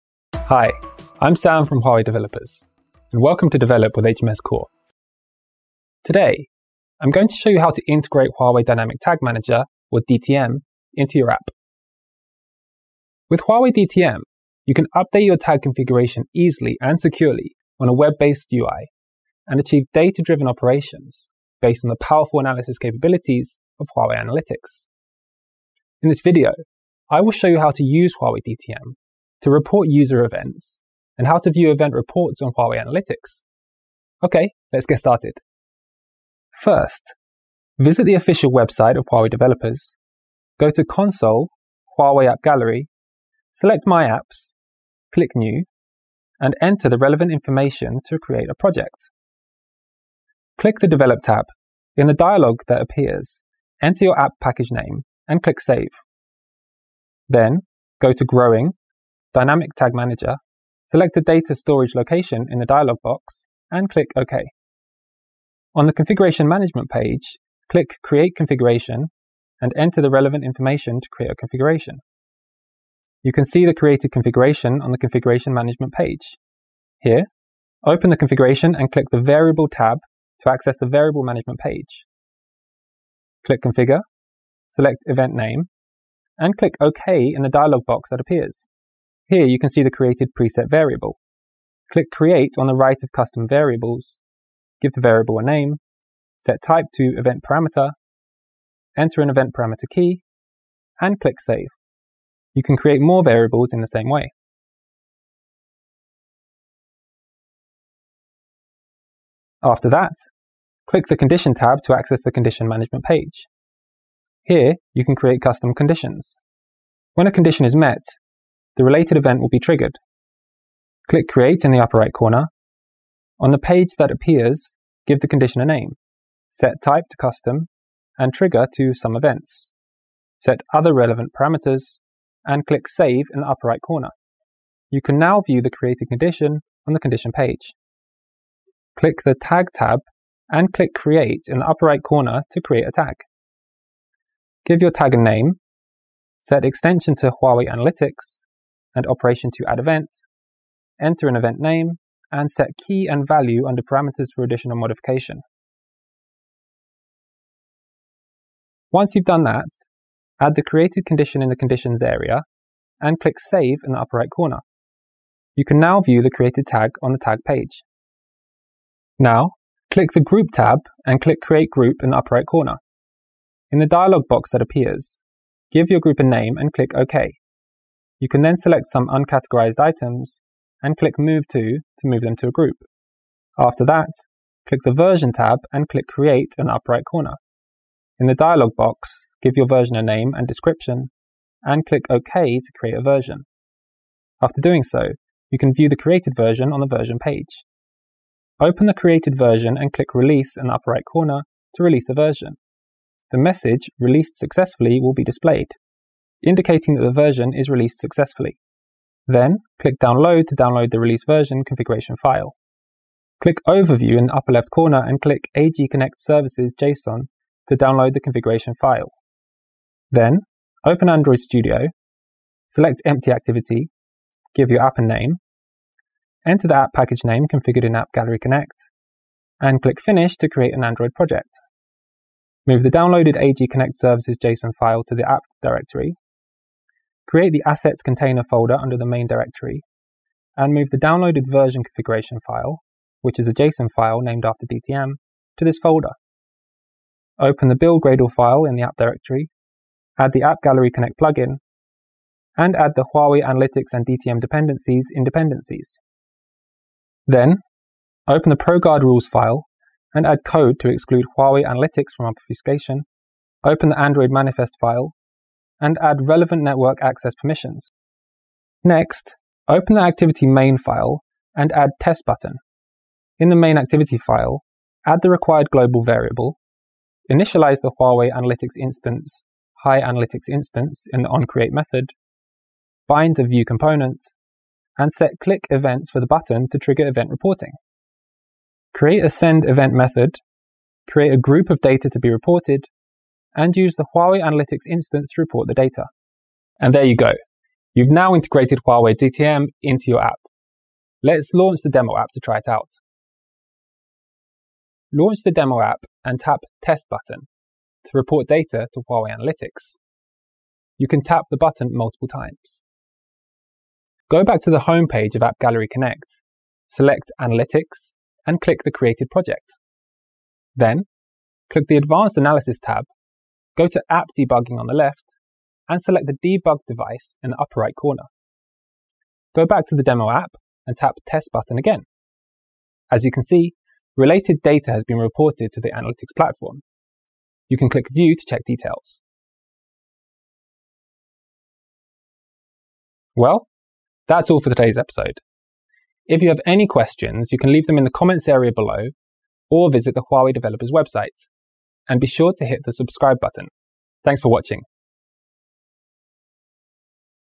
HMSTips: How to integrate with HUAWEI Dynamic Tag Manager
This video introduces the functions and advantages of HUAWEI DTM, and how to integrate the DTM SDK into applications.

Also, you’ll see how to process events and report them to Huawei Analytics Platform in developed applications.